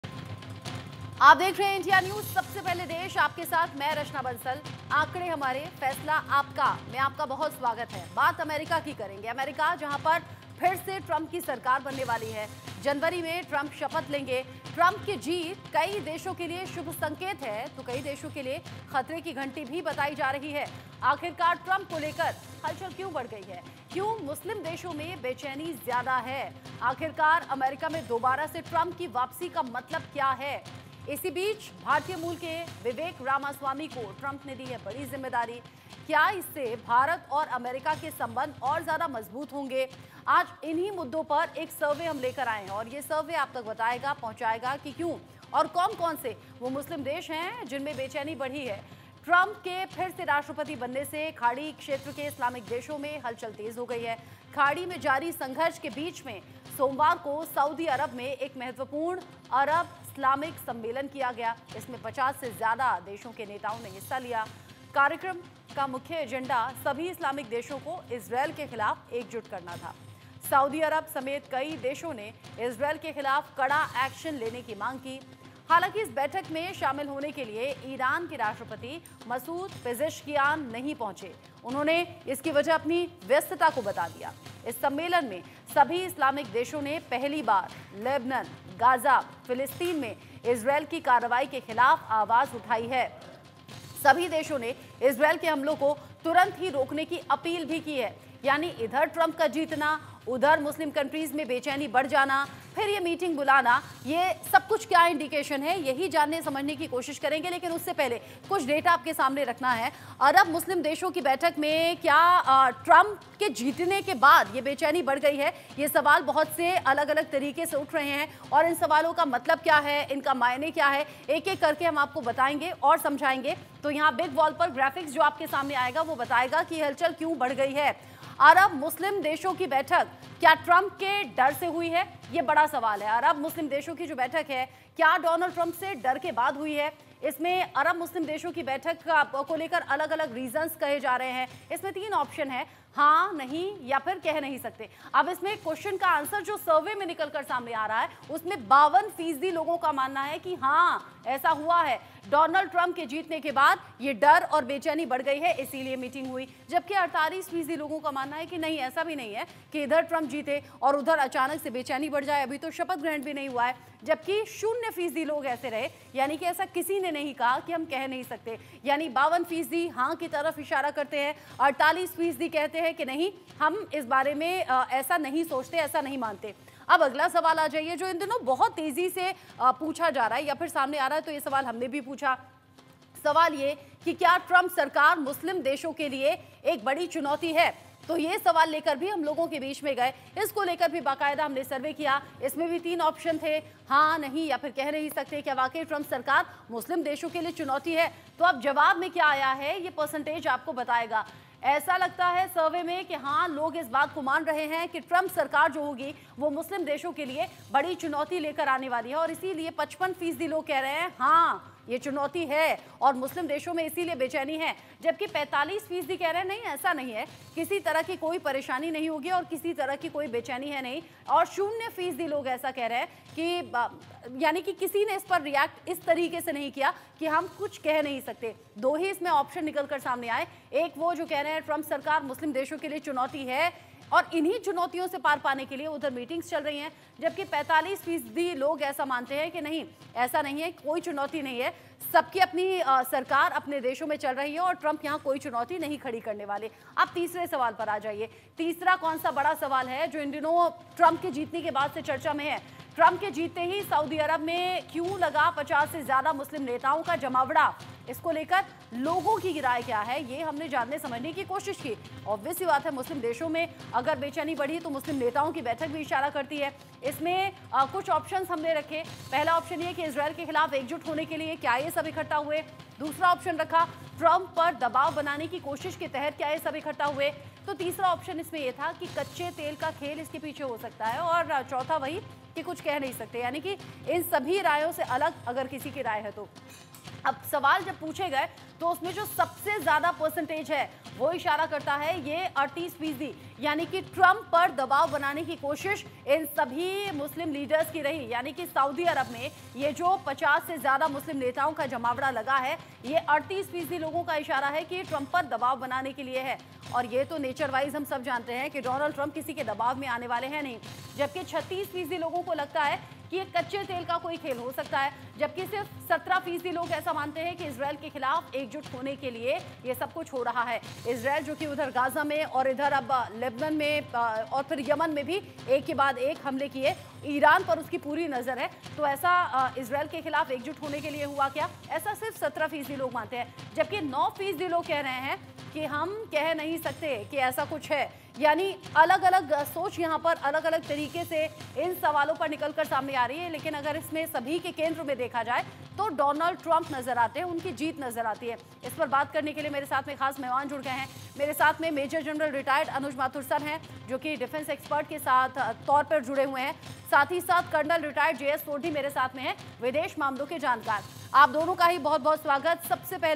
Muslim Countries on Donald Trump: क्या ट्रंप सरकार मुस्लिम देशों के लिए चुनौती है ? | India News
डोनाल्ड ट्रम्प के व्हाइट हाउस में फिर से प्रवेश को पूरी दुनिया में, खासकर दुनिया भर के मुस्लिम देशों में, बेसब्री से देखा जा रहा है, क्योंकि दुनिया यह देखने के लिए इंतजार कर रही है कि उनका वर्तमान राष्ट्रपति पद किस तरह से चलेगा। क्या यह पिछले राष्ट्रपति की तरह 'अमेरिका फर्स्ट' होगा या विशेष रूप से मुस्लिम देशों के साथ व्यवहार करते समय अधिक सहिष्णु होगा? डोनाल्ड ट्रम्प ने चार साल की अनुपस्थिति के बाद अमेरिकी राष्ट्रपति पद पर पुन: कब्ज़ा कर लिया है। वह 47वें अमेरिकी राष्ट्रपति बन गये हैं और उन्होंने अमेरिकी हितों को प्राथमिकता देने और अन्य देशों की ताकत और संरेखण के आधार पर गठबंधन का आकलन करने के अपने दृष्टिकोण पर लौटने का संकेत दिया है।

Donald Trump's re-entry into the White House is being keenly watched by the whole world, especially in Muslim countries across the globe, as the world waits to see how his current presidency will play out. Will it be 'America First' like the previous president or will it be more tolerant especially when dealing with Muslim countries? Donald Trump has re-occupied the US presidency after an absence of four years. He has become the 47th US President and has indicated a return to his approach of prioritising US interests and assessing alliances based on the strength and alignment of other countries.